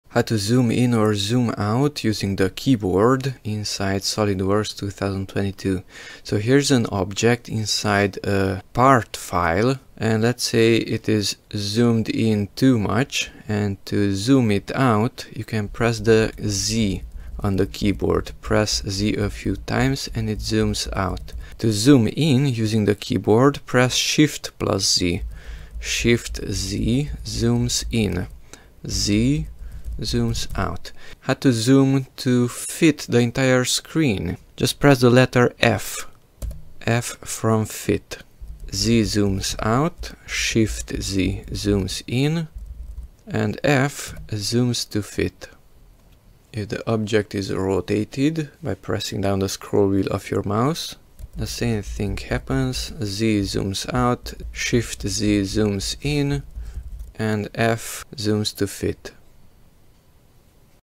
SolidWorks Zoom keyboard shortcuts (Zoom IN, Out, Fit)
Z = Zoom Out,
Shift + Z = Zoom In,
F = Zoom to Fit